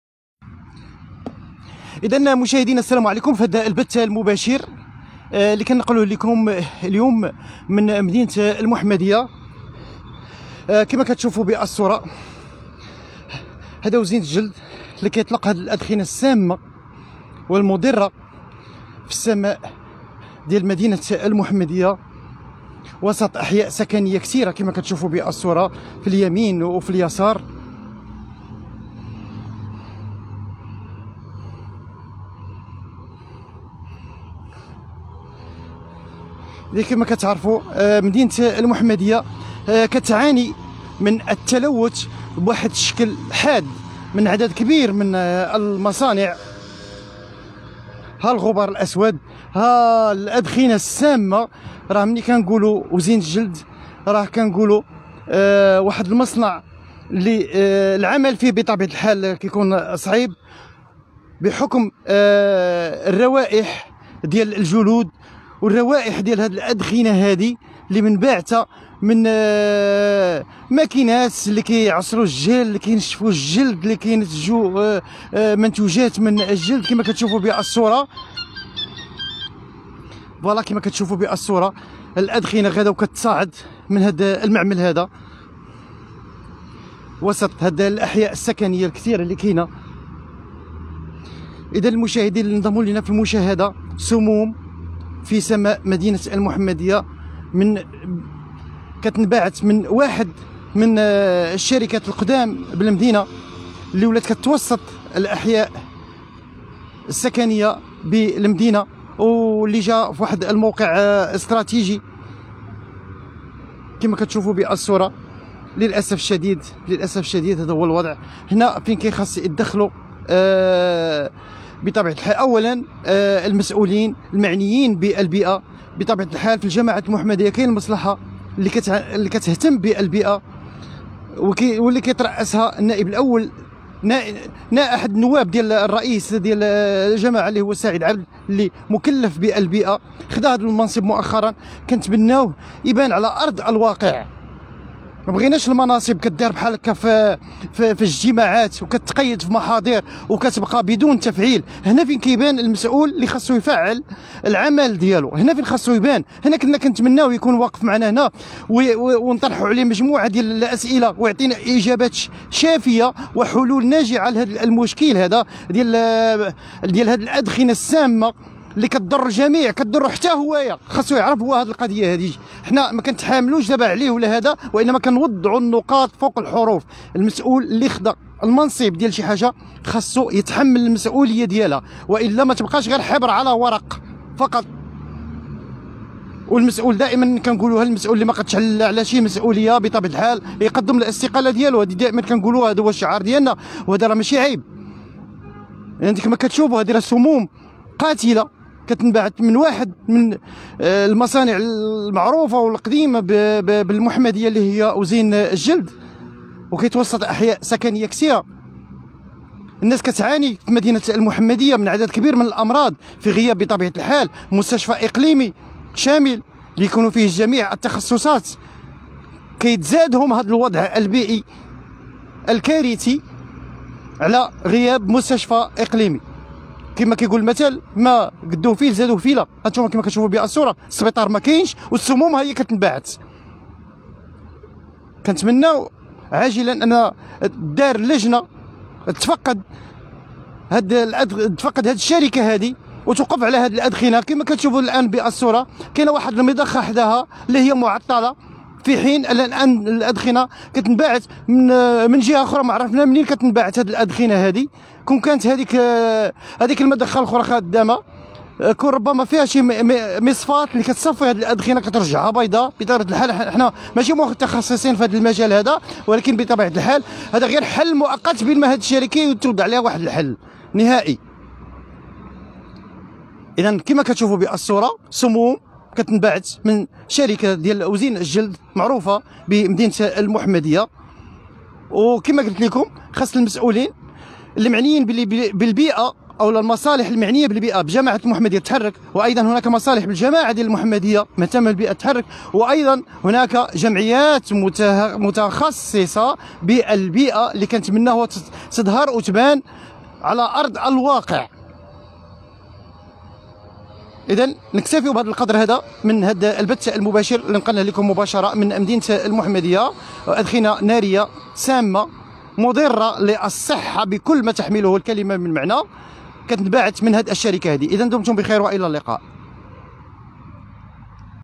شوفو على سموم من الأدخنة المضرة تجتاح سماء المحمدية من وزين الجلدشوفو اش واقع
شوفو على سموم من الأدخنة المضرة تجتاح سماء المحمدية من وزين الجلد
شوفو اش واقع